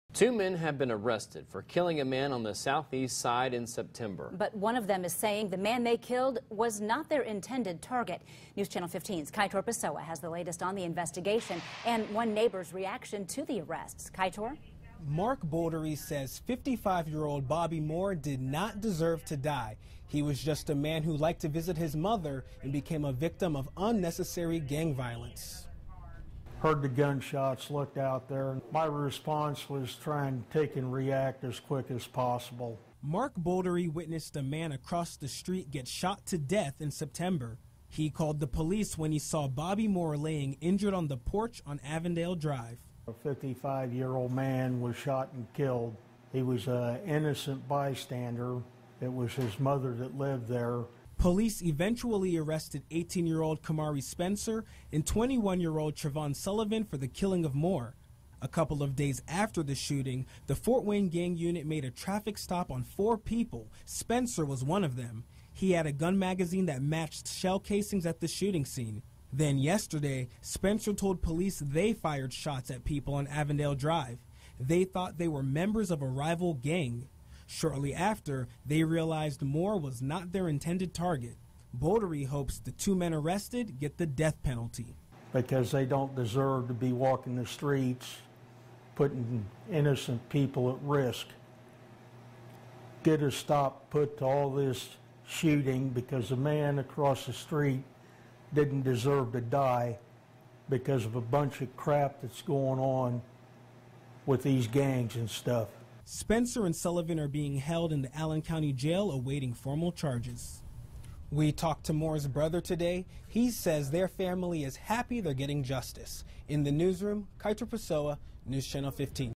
Police: Gang member says he killed the wrong man
According to a court document, the suspect told police he and another person shot a 55-year-old Fort Wayne man to death back on September 20 because they thought was a rival gang member.